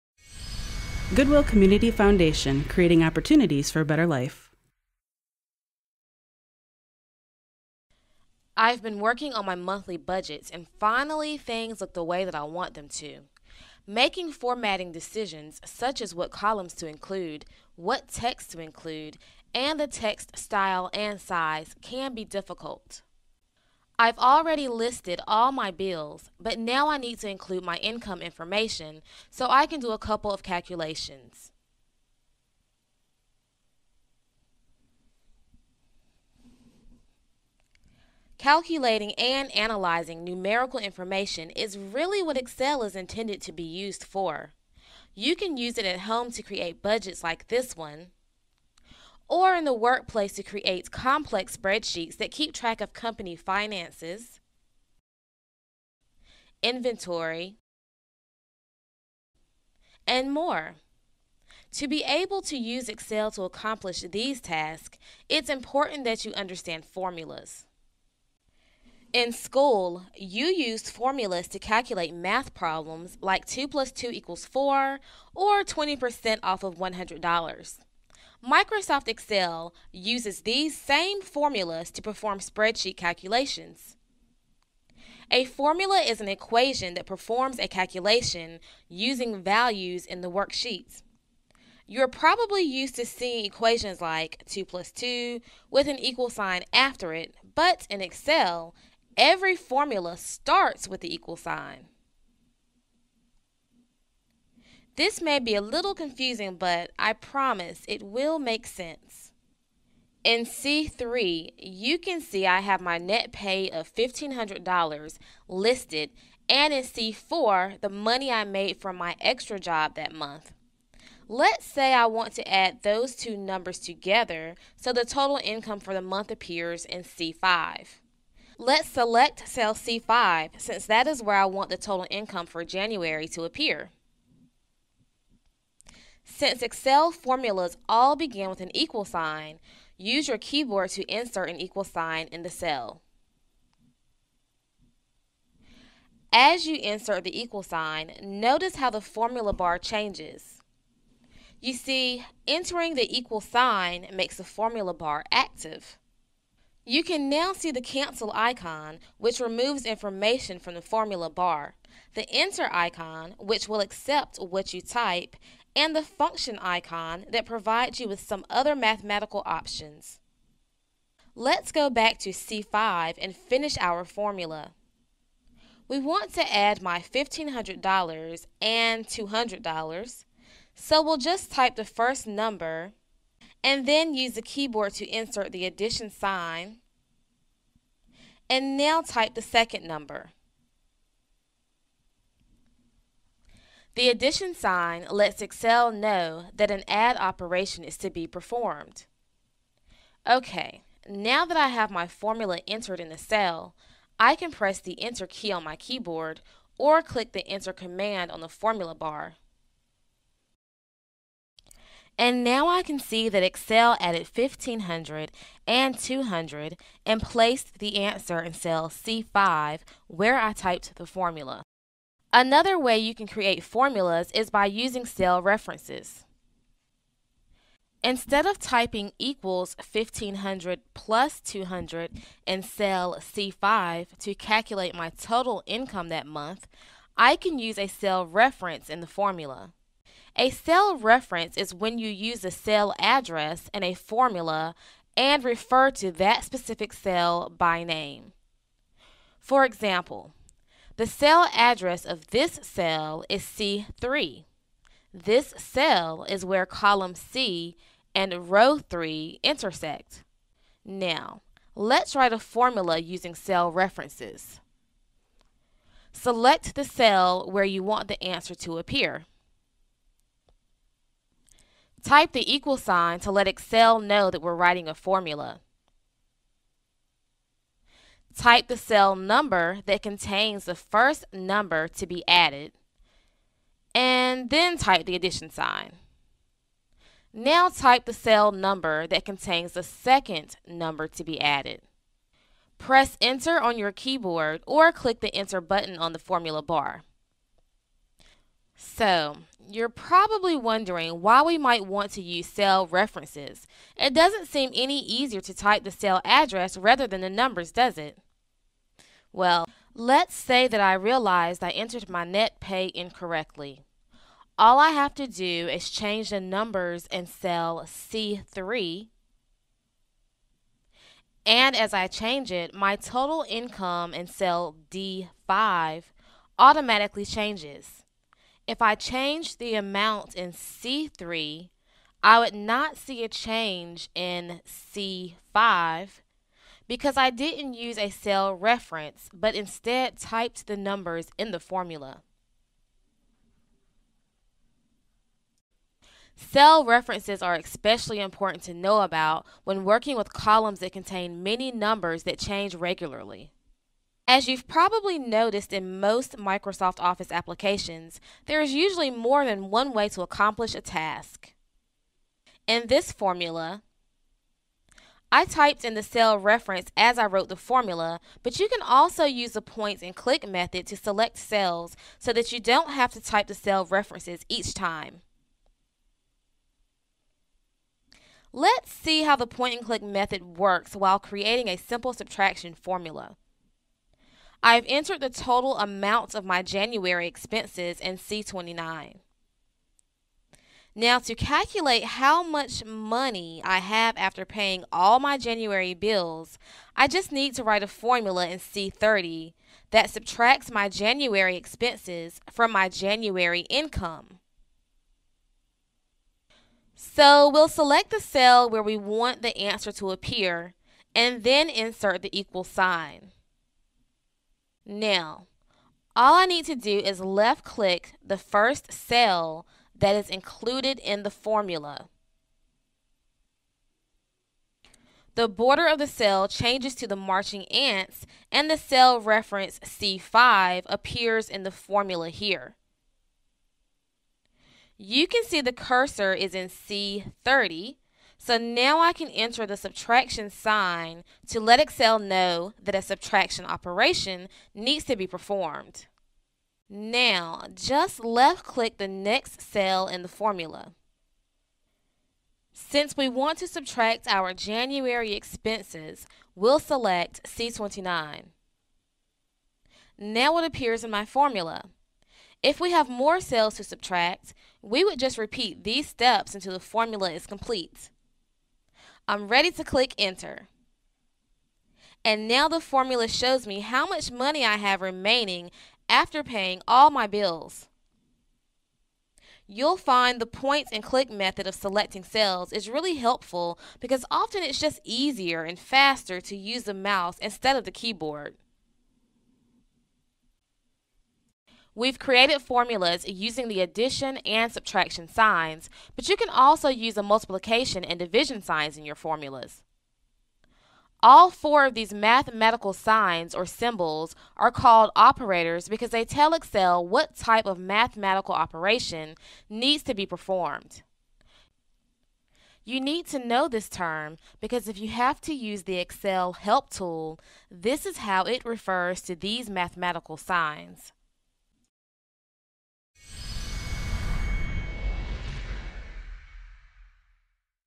Excel 2007: Formulas
In this video, you’ll learn more about creating formulas in Excel 2007. for our text-based lesson.

This video includes information on:
• Creating simple formulas that add two numbers
• Creating simple formulas using the point-and-click method
• Creating simple formulas that add, multiply, or divide the content of two cells
• Using cell references